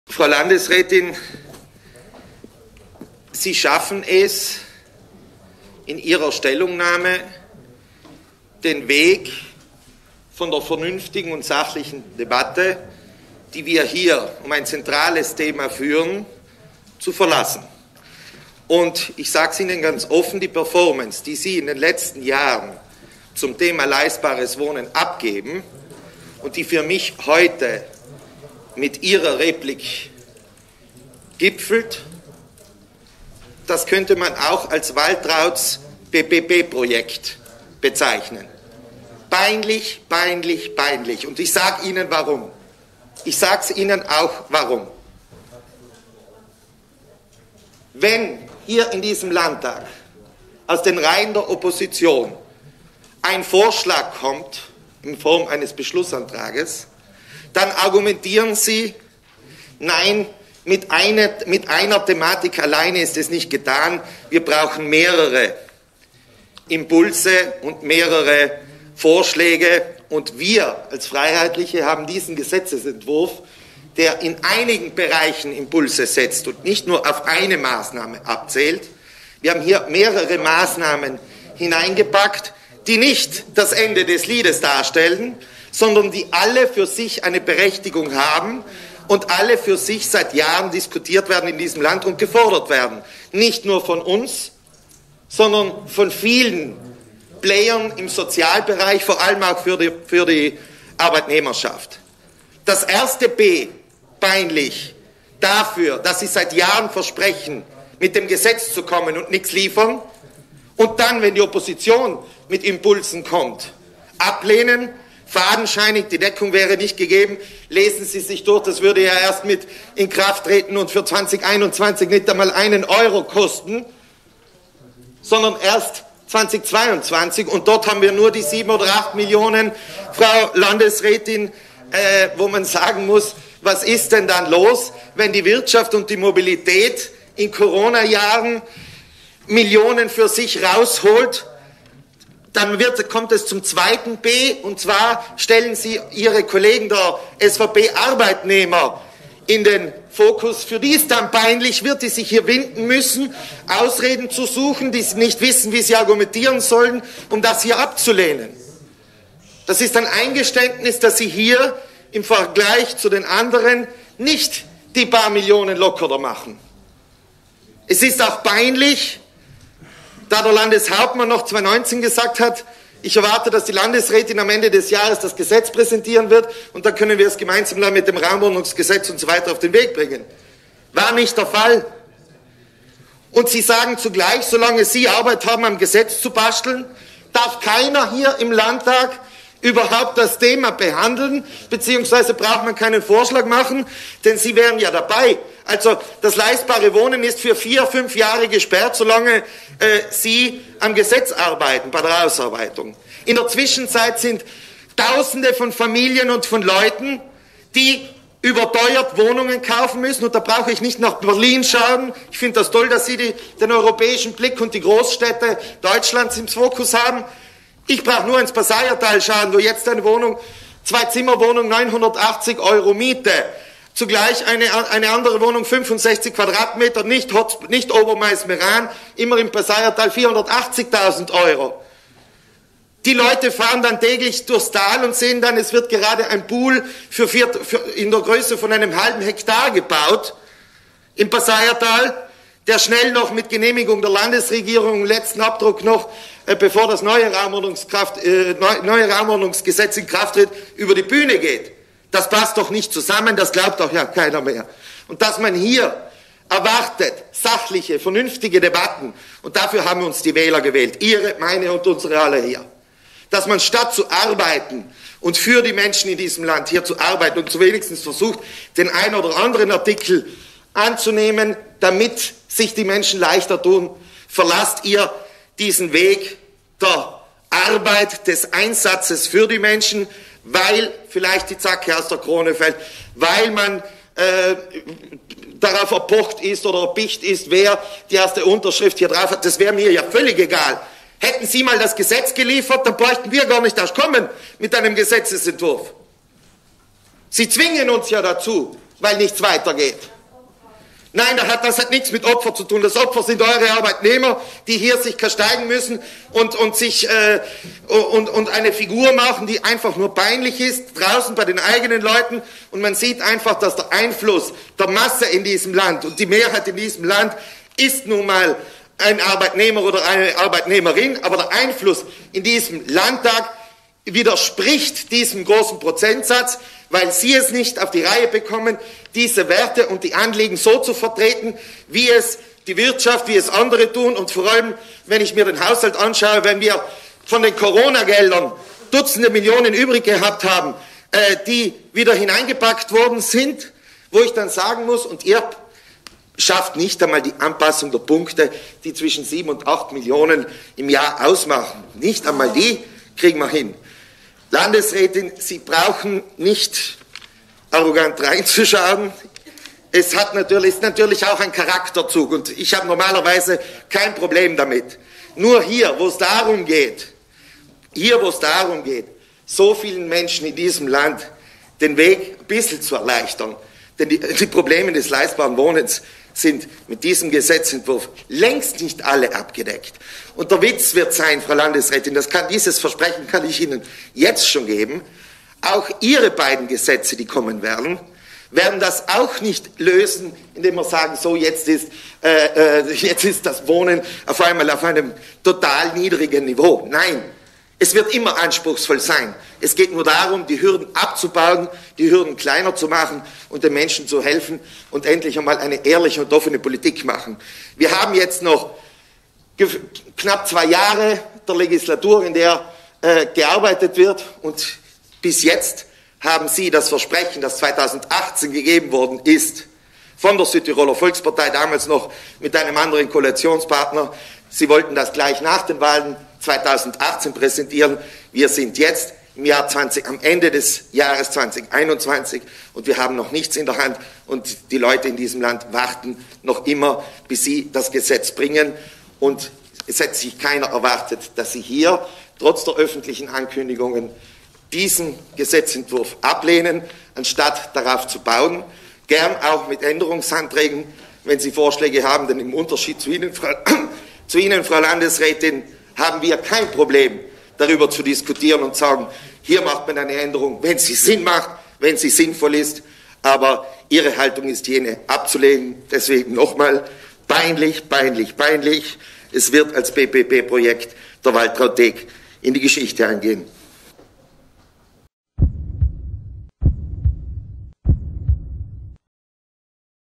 Leiter Reber: ❝Deeg liefert Phrasen, statt Maßnahmen für bezahlbares Wohnen!❞
SVP und Lega versenken Gesetzentwurf zum leistbaren Wohnen – Opposition geschlossen dafür

Von der Punkteanpassung in der Wohnbauförderung bis zum sorglosen Vermieten: Der von den Freiheitlichen vorgelegte Gesetzesentwurf hat mehrere Maßnahmen vorgesehen, die das Wohnen in Südtirol bezahlbarer machen sollen. Doch SVP und Lega haben das Gesetz noch vor der Debatte zu den einzelnen Artikeln und Vorschlägen versenkt. Der freiheitliche Fraktionssprecher Andreas Leiter Reber bezeichnet die Performance der SVP in Sachen Wohnen als „peinliche und nicht enden wollende Ankündigungspolitik“, deren Versäumnisse schwer auf Südtirols Jugend, Familien und den Mittelstand lasten würden.

„Dass heute Vertreter der SVP-Arbeitnehmer uns inhaltlich recht gegeben, aber am Ende gegen unseren Gesetzesentwurf gestimmt haben, ist bezeichnend für die aktuelle Volkspartei, in der das Ressort Mobilität und mehrere Wirtschaftslobbys einen für das Land ungesunden Raum einnehmen. Vor vier Jahren hat die Volkspartei das „Leistbare Wohnen“ zur obersten Priorität ihrer Sozialpolitik erklärt. Das Versprechen von Landesrätin Waltraud Deeg, noch im Jahr 2018 dem überteuerten Südtiroler Wohnraum gesetzlich entgegenwirken zu wollen, ist sie bis heute schuldig geblieben“, so Leiter Reber.

„Bereits 2019 hat unsere Fraktion einen umfassenden Gesetzentwurf zum leistbaren Wohnen ausgearbeitet, über den in der zuständigen Kommission ausführlich beraten und der laufend aktualisiert wurde. Neben der Anpassung der Punkte für die Wohnbauförderung, der Wiedergewinnung von leerstehenden Wohnungen und Anreizen für das sorglose Vermieten, hat der Gesetzesentwurf auch Erleichterungen für Wohnbaugenossenschaften vorgesehen. Allesamt Maßnahmen, auf welche viele Südtiroler seit Jahren warten und die von der SVP als Wahlkampfthemen missbraucht werden. Und zwar zweimal, denn bevor die Versprechen aus dem Jahr 2018 konkret umgesetzt werden, stehen wieder die nächsten Landtagswahlen an“, so die Freiheitlichen.